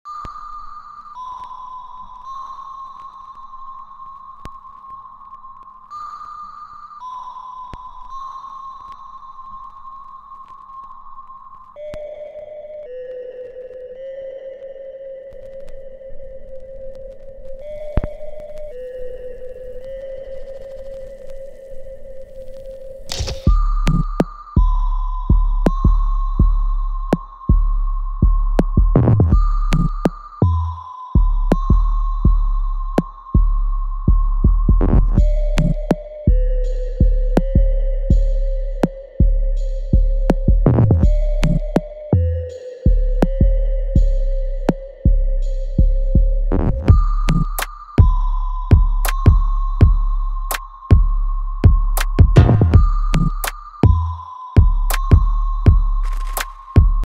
PEUGEOT 206 CC CLUTCH KIT
A quick look over a clutch kit replace ... on a 206 cc 1.6 16v full LuK clutch kit .
We offer,  clutch replacement,  car servicing,  brake discs and pads service,  exhaust system,  battery replacement,  new and used car parts...